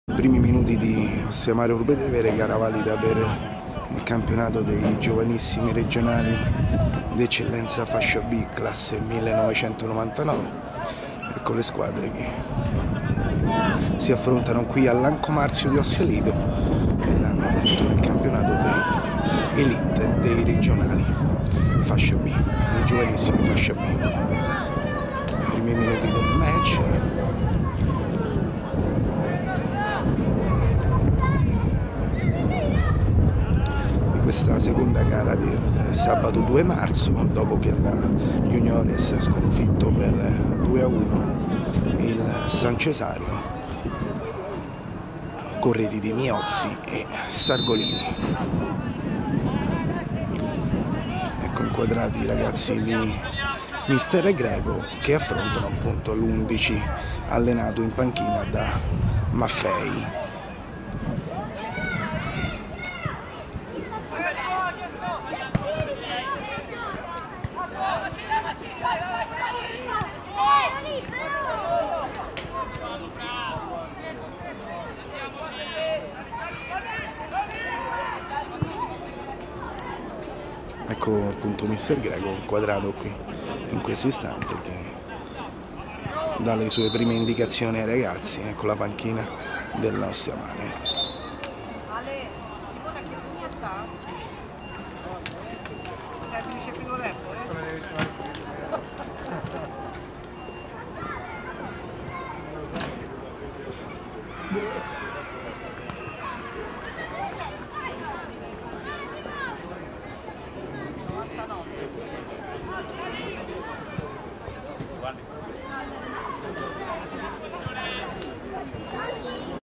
Ostiamare-Urbetevere 1-1 (Giovanissimi Elite Fascia B)
Le prime immagini del match pomeridiano tra i Giovanissimi Elite Fascia B dell'Ostiamare, di mister Greco, e quelli dell'Urbetevere di Maffei. La gara si è chiusa sull'1 a 1 con reti di Viti per gli ospiti e Carrera per i biancoviola